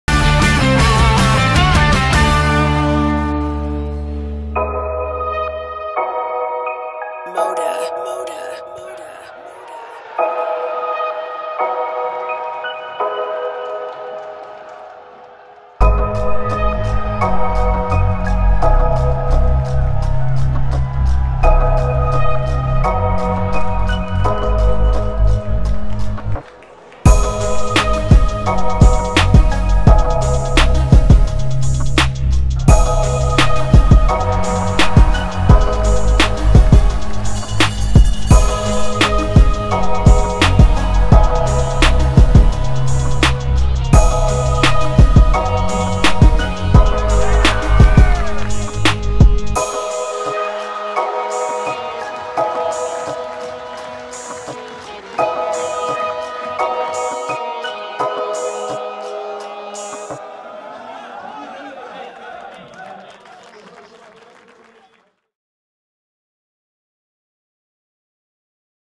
De hoogtepunten van speelronde 33 in vogelvlucht
De mooiste momenten van speelronde 33 in de Tweede Divisie.